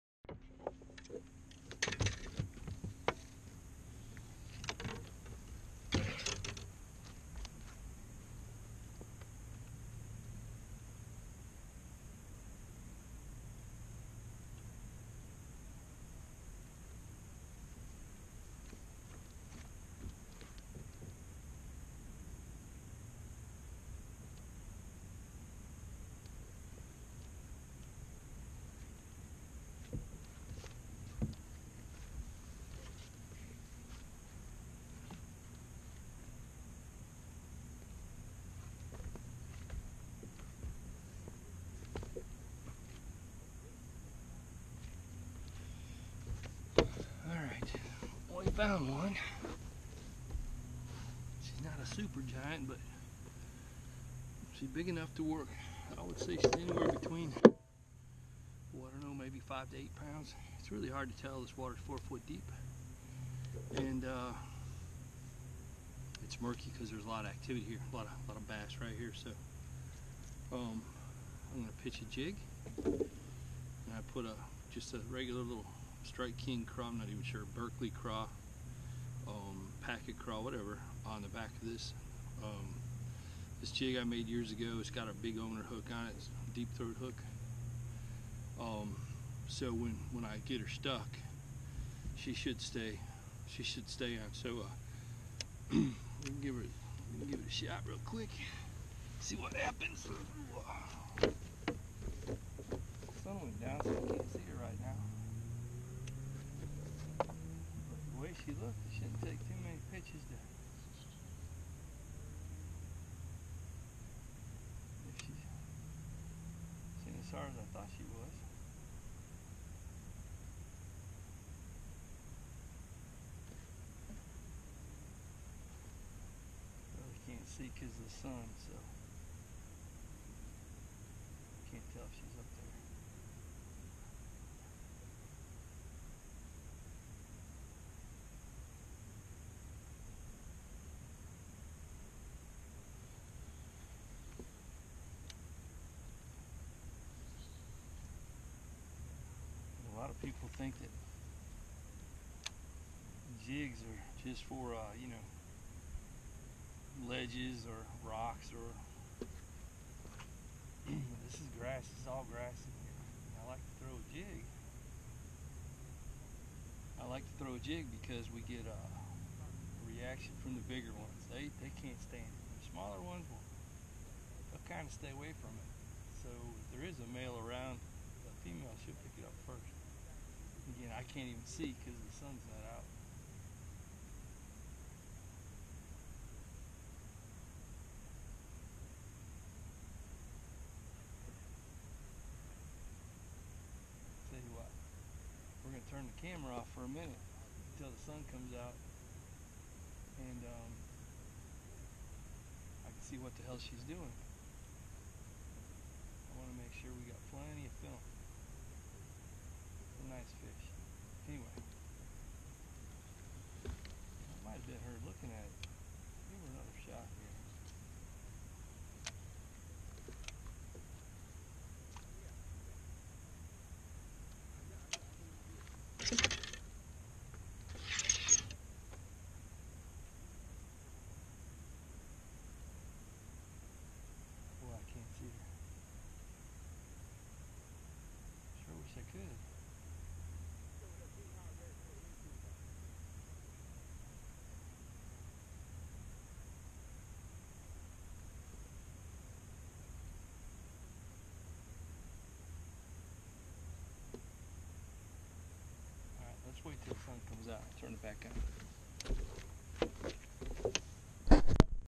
How to catch bass in Florida
Video on fishing for largemouth bass in Florida's Big Bend